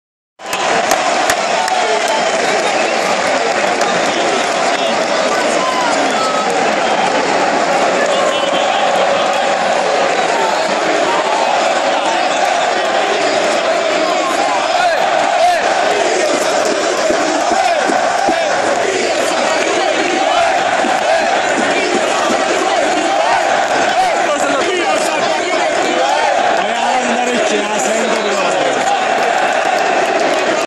Napoli - Juventus (26/09/15)...e il saluto alla Curva B
Il Napoli vince con la Juventus e a fine partita gli azzurri salutano anche la Curva B